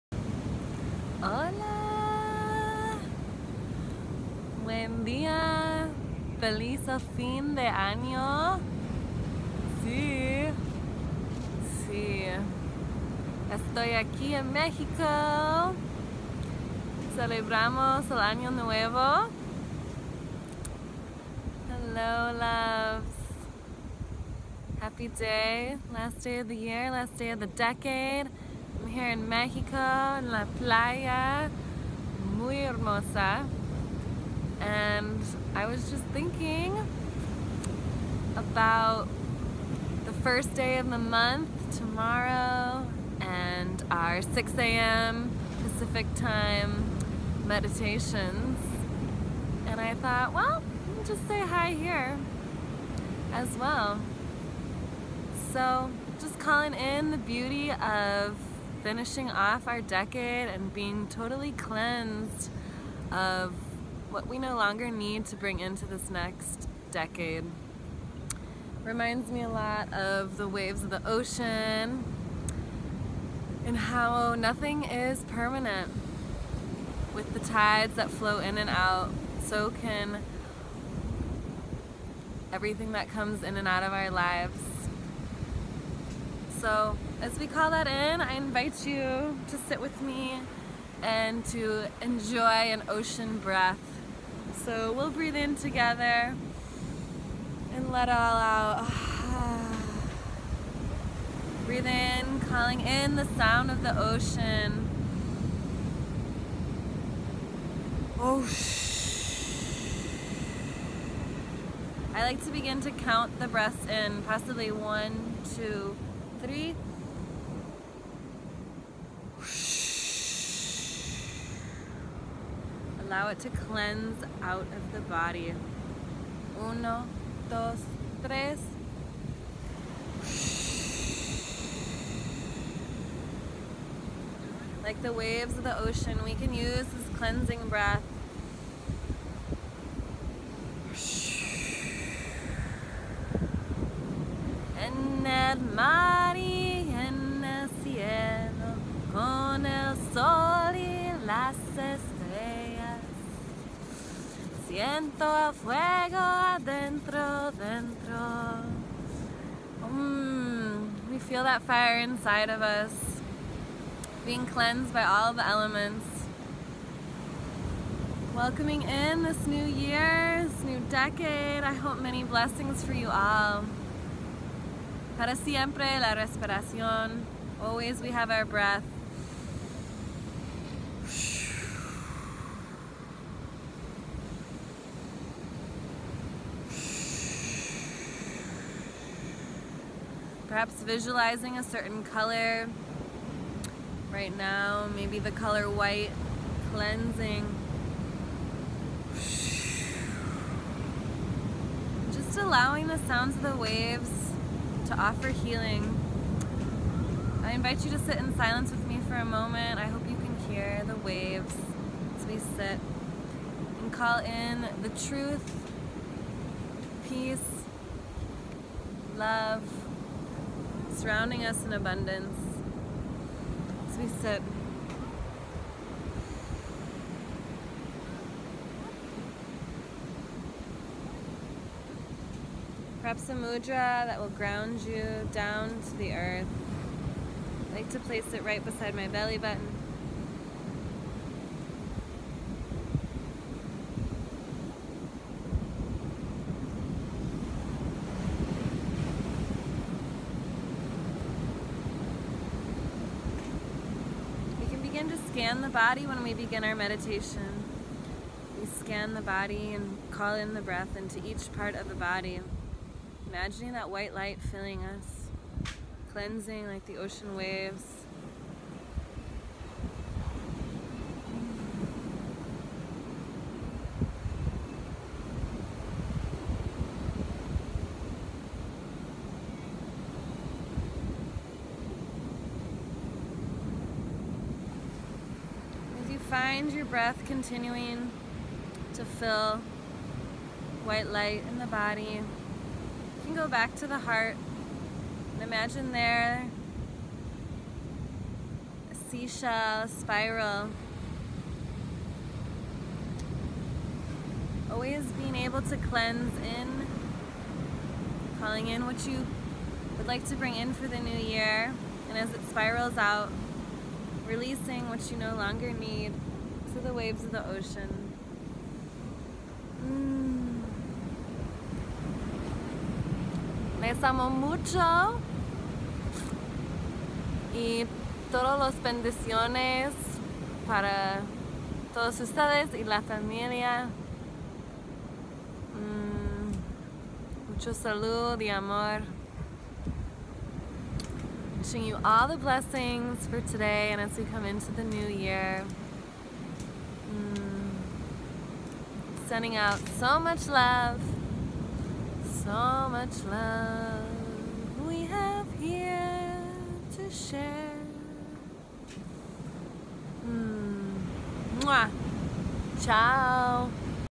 Moment of Meditation 12/31/19
Feliz año nuevo!! Feliz década nueva!!!
Bendiciones desde la playa en México por todo el salud, amor, y días felices!
Joining you for a Meditation cleansing with respiración del océano and visualizing pure love white light of truth cleansing our bodies. Spiral out what we no longer need to take with us mentally, emotionally, physically, spiritually into the next decade, next year.

2020 gateway full of many blessings of abundance for you and yours! For the healing of our Mother Earth, peace and justice for all.

Salud y Amor,
Brittany